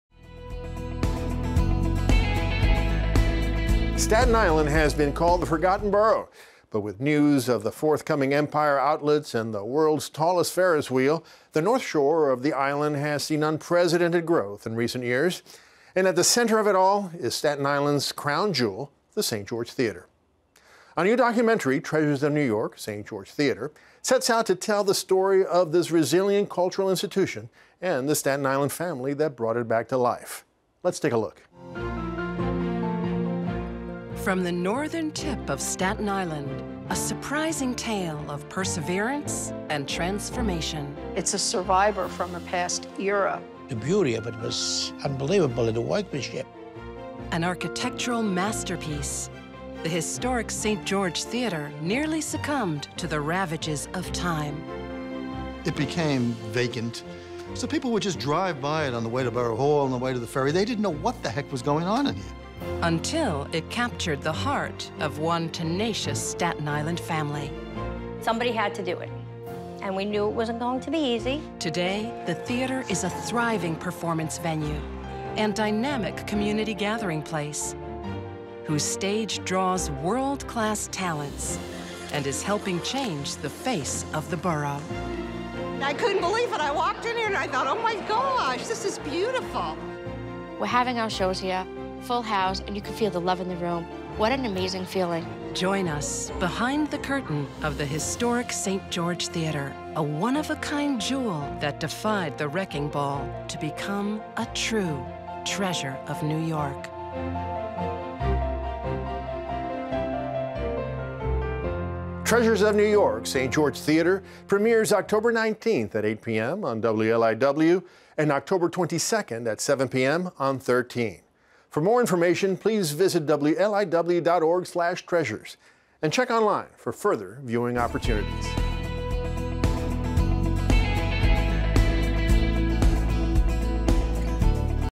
A Staten Island Treasure
Once beloved, then forgotten, it sat like the borough it resides in, an oft-afterthought.  Now the St. George Theatre in Staten Island is truly a “Treasure of New York,” as is the family who gave life to this once dilapidated landmark.  We've got your glimpse inside it's majesty.